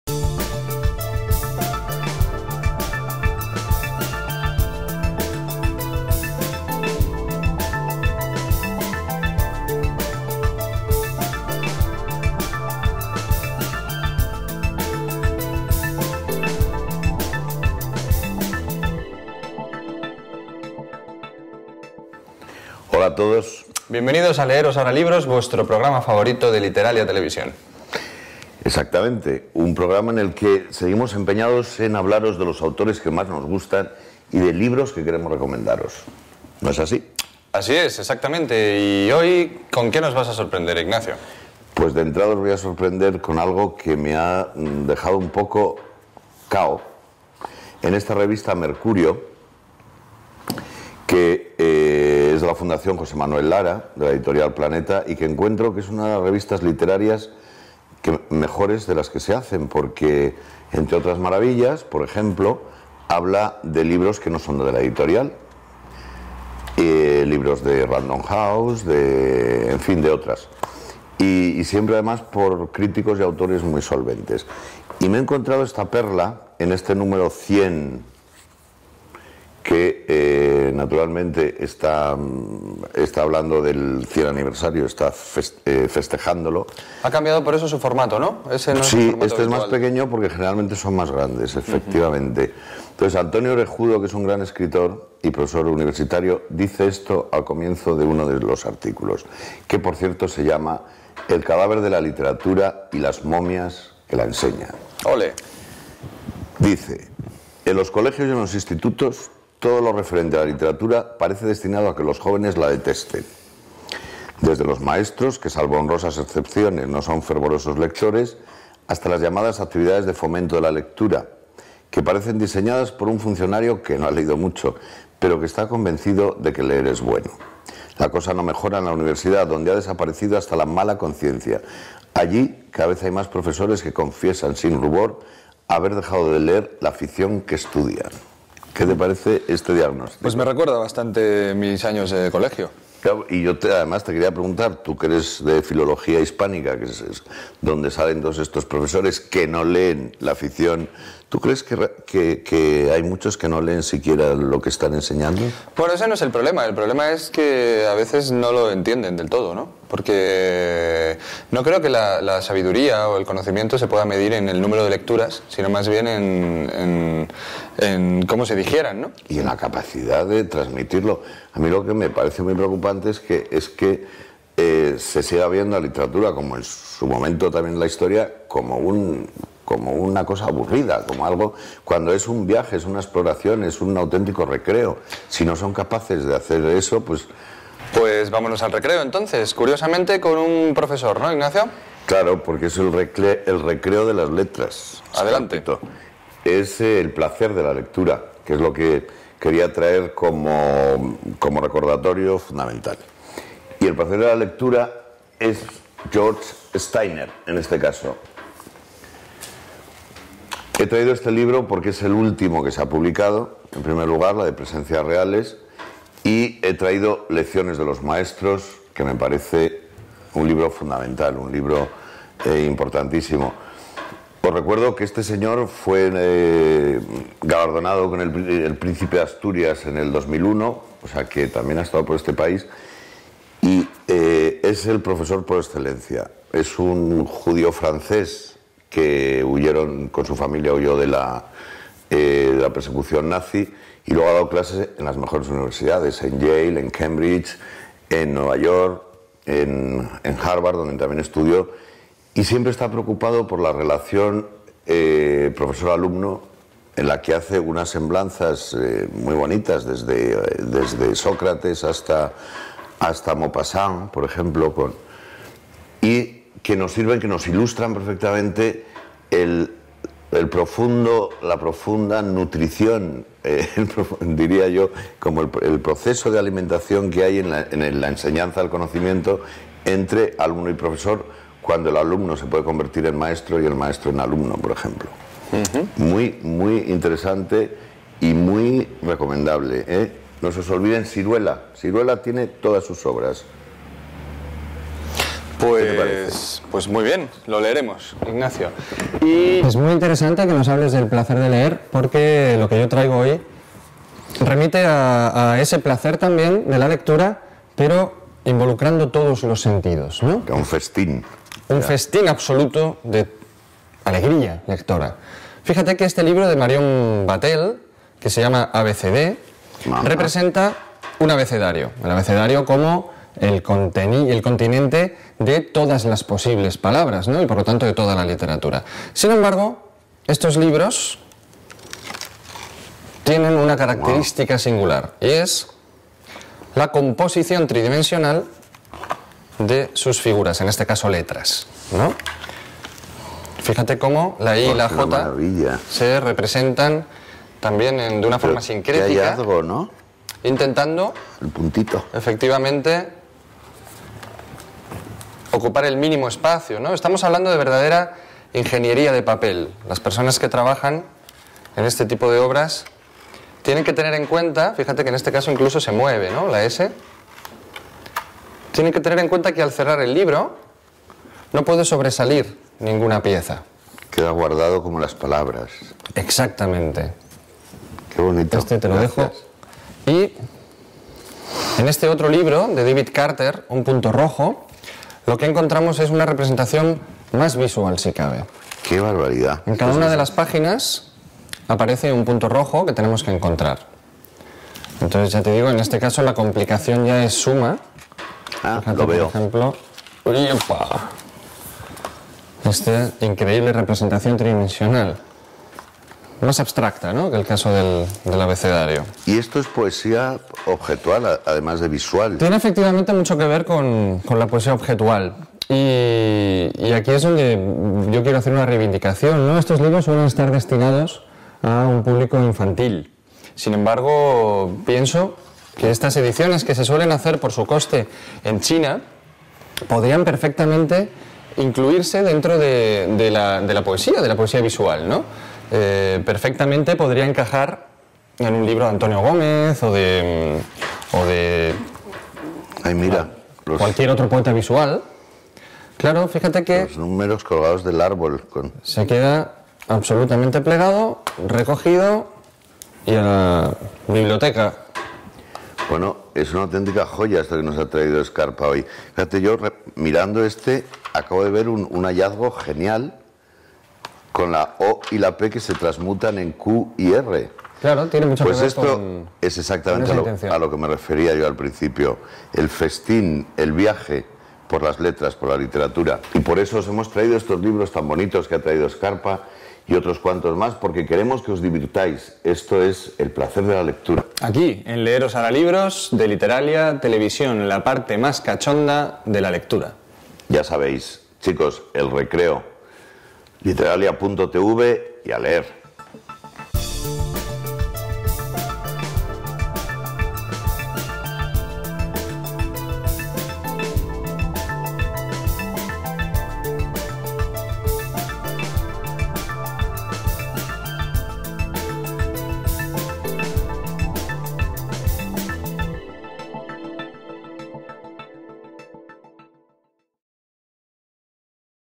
7º Leer Os Hará Libros - Pedagogía magistral de George Steiner y Poesía de la Imagen
Según Antonio Orejudo, el aprendizaje de la Literatura en España es un despropósito a menudo imputable a los profesores que ni siquiera leen las obras que enseñan. Como antídoto a este panorama desalentador, Merino habla en este capítulo de George Steiner como el pedagogo por excelencia y de la magnífica revista literaria Mercurio que ha celebrado su número cien con una edición especial. Gonzalo Escarpa, por su parte, trae dos libros de contenido visual con un indiscutible aroma poético.
George Steiner [París, 1925]: Como Stefan Zweig, era hijo de judíos vieneses aunque nacido en París y emigrado a Estados Unidos tras la invasión nazi de Francia. Estudió en Harvard y ha desarrollado una brillante carrera académica en Gran Bretaña e Italia. Gran especialista en literatura comparada, ejerció la crítica literaria en la prestigiosa revista New Yorker. Autor de relatos breves e incluso de una novela, su genio brilla en ensayos como Tolstoi o Dostoievski, La muerte de la tragedia, Gramáticas de la creación o Lecciones de los maestros, libro fundamental en el que desarrolla el tema de la relación ética profesor-alumno. En 2001 obtuvo el Premio Príncipe de Asturias de Comunicación y Humanidades.
Libros visuales: Tanto El punto rojo como ABCD son libros hechos con desplegables de magnífica factura y exquisita composición. Tan complicados de producir en tiradas sustanciales que sólo pueden fabricarse en China, según asegura Gonzalo Escarpa, para que sean rentables. El primero exhibe un conjunto de piezas magníficas, inspiradas en grafías y símbolos sencillos, en las que siempre aparece escondido un punto rojo que debe encontrarse. El segundo contiene un alfabeto de inspiración dadaísta, con alto valor estético y hallazgos espectaculares tanto formales como semánticos. Ambos libros, aunque enfocados en principio hacia un público infantil, pueden muy bien convivir con la poesía visual más contemporánea.